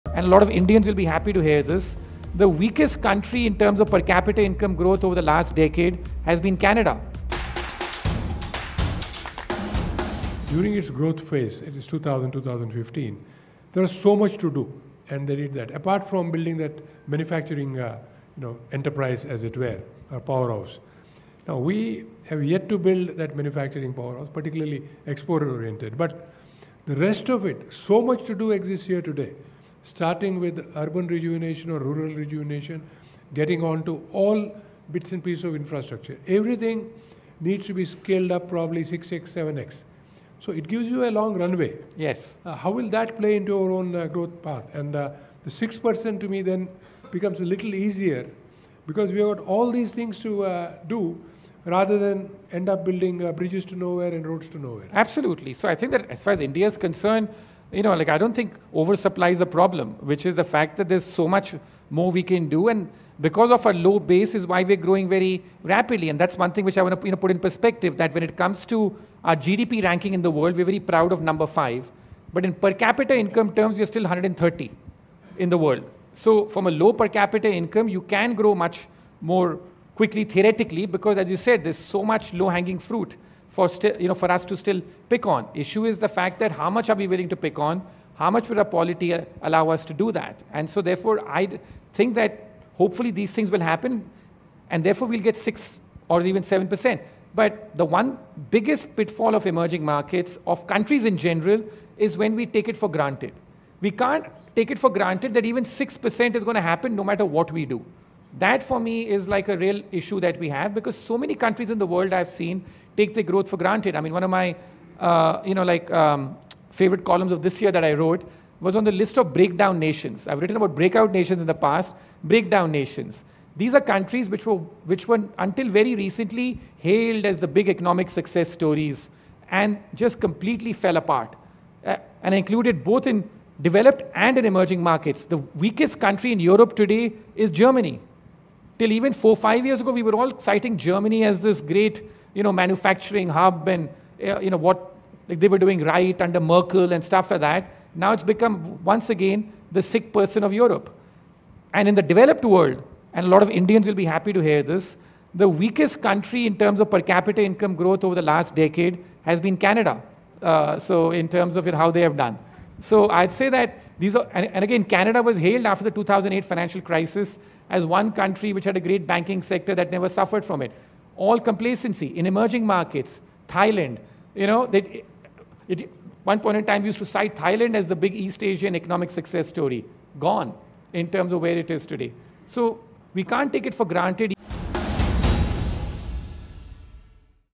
Ruchir Sharma on Breakdown Nations: Lessons from Germany and Canada | Express Adda
Ruchir Sharma highlights the recent struggles of economies once hailed as success stories, including Germany and Canada. He explains how these countries, despite their strong positions just a few years ago, have now become some of the weakest performers in the developed world. Sharma draws attention to the dangers of complacency and the importance of not taking economic growth for granted, using these examples to caution other nations, including India, against overconfidence.